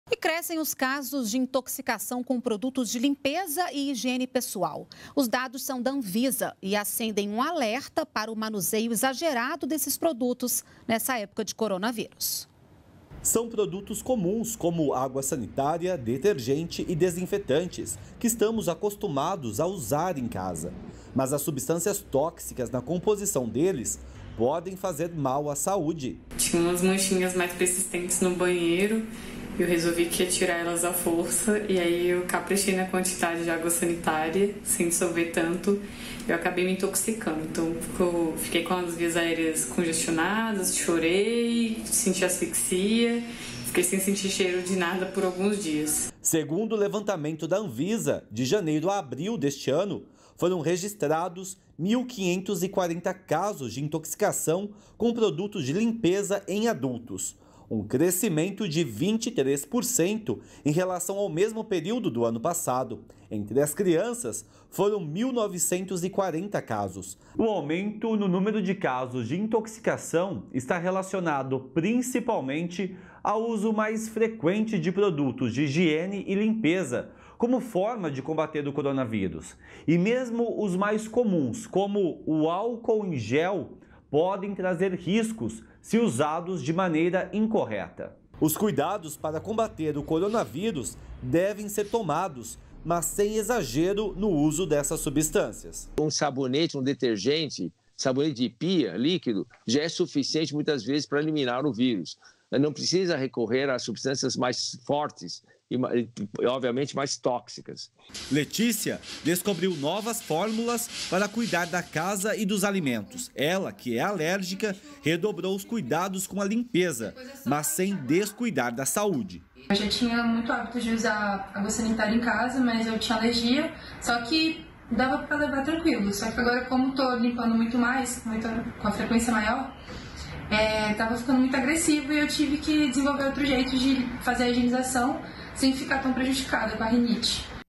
Mais casos de intoxicação com produtos de limpeza e higiene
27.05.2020 - Cresce o número de casos de intoxicação com produtos de limpeza e higiene pessoal devido ao uso mais frequente durante o isolamento social. Os dados são da Anvisa.